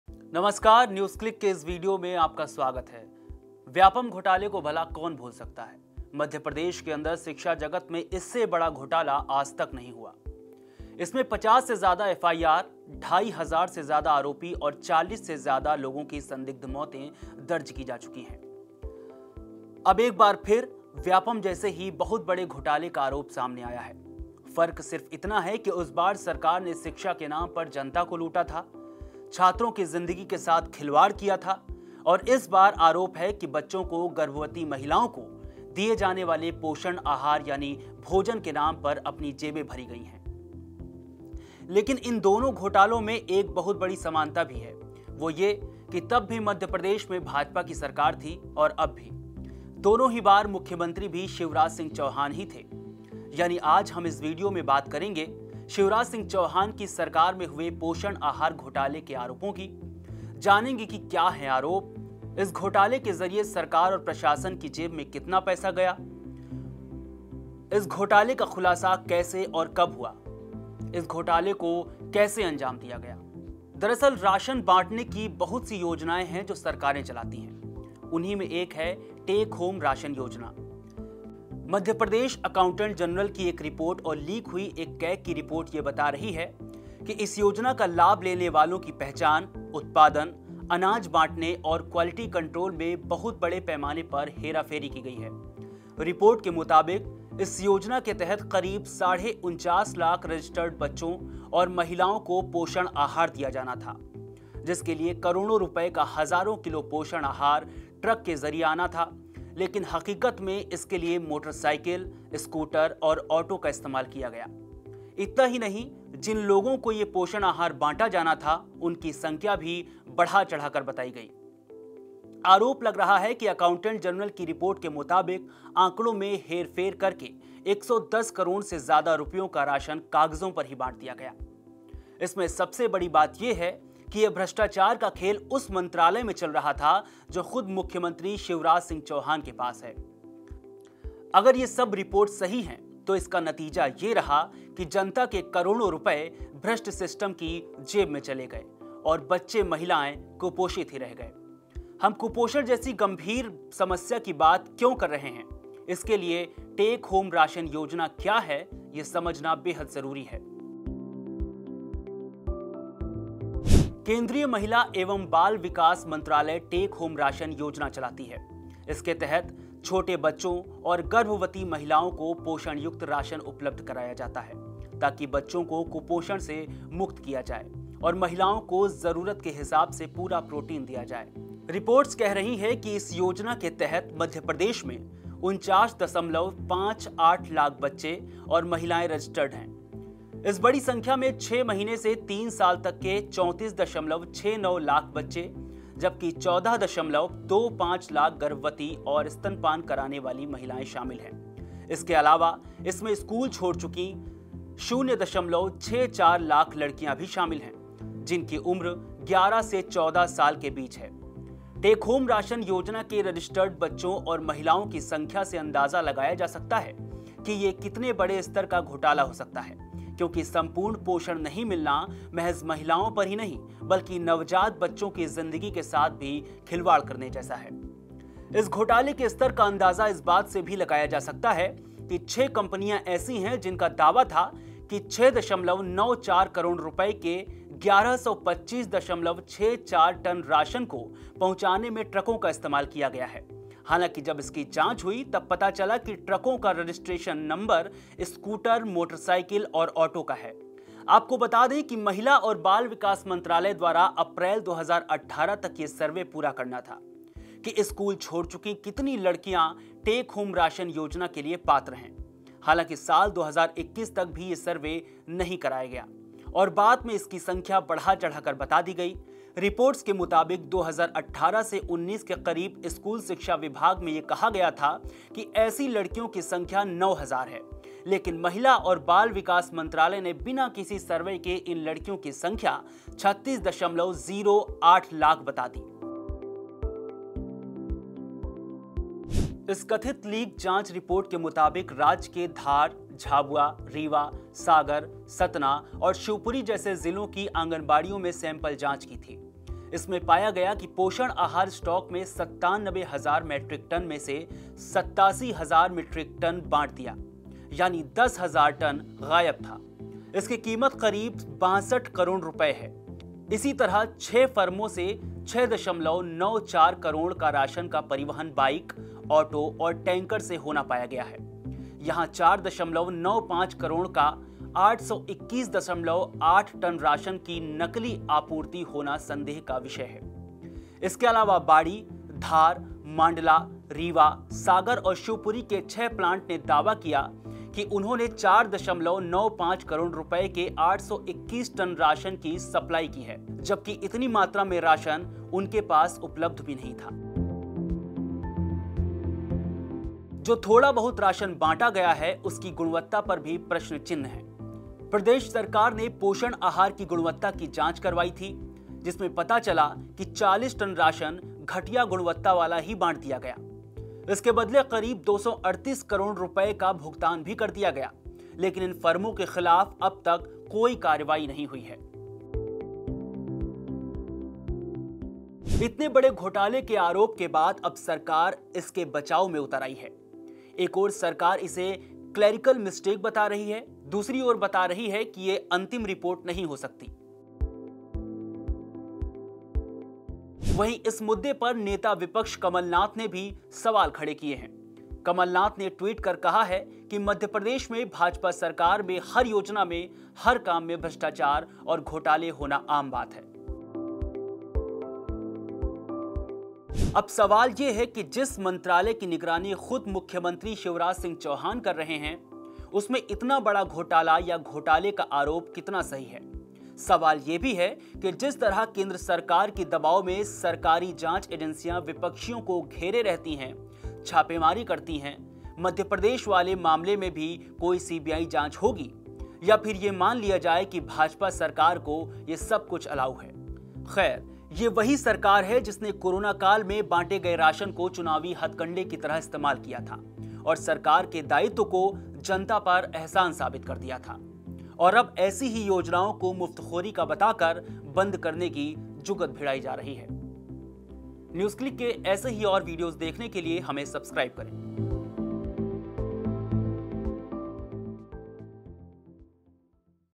कुपोषण के ख़िलाफ ‘Poshan Aahar Yojna’ में घोटाला!
मध्य प्रदेश में हुए ‘पोषण आहार घोटाले’ ने Vyapam घोटाले की याद दिला दी। कुपोषण ख़त्म करने के लिए बच्चों और गर्भवती महिलाओं को दिए जाने वाले पोषण आहार का ट्रांसपोर्टेशन और गुणवत्ता समेत तमाम चीज़ें हेरा-फेरी का शिकार हो गईं। घोटाले का कारण क्या है? सरकार और प्रशासन की जेब में कितना पैसा आया है? ये घोटाला कैसे सामने आया? इस वीडियो में विस्तार से देखिए